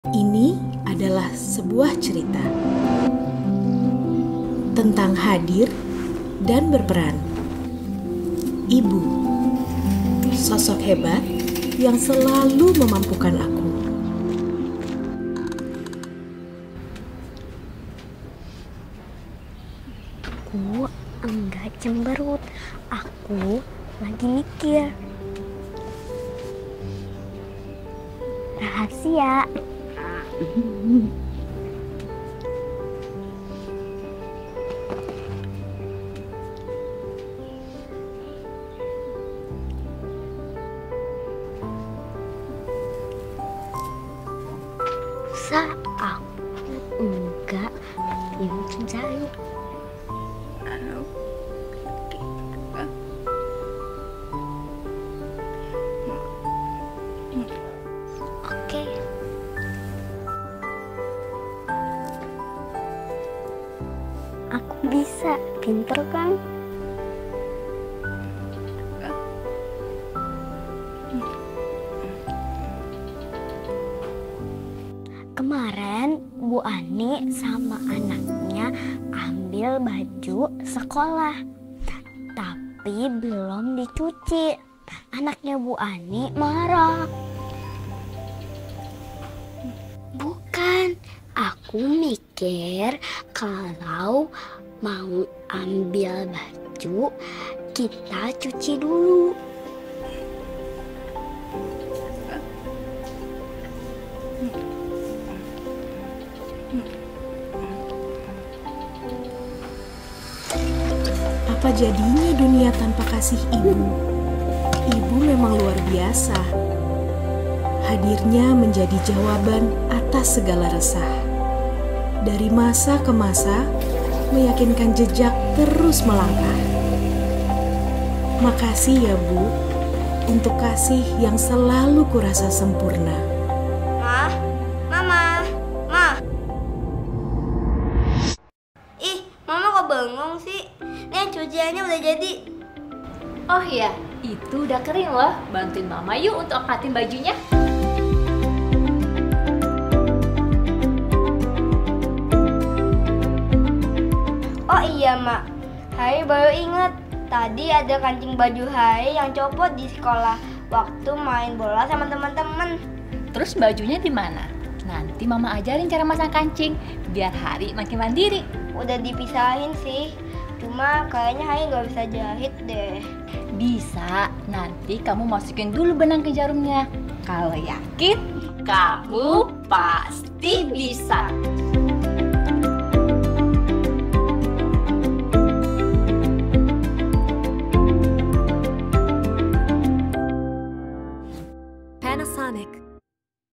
Panasonic PASTI Untuk Indonesia dari Masa ke Masa
Ini adalah kisah tentang Hadir dan Berperan seorang Ibu yang bermakna bagi anaknya.

Pelajaran yang Terang dapat sangatlah bermakna baginya sehingga meyakinkan jejaknya terus melangkah❤

Seperti Mesin Cuci Panasonic, PASTI akan selalu ada bagi keluarga Indonesia dari masa ke masa✨️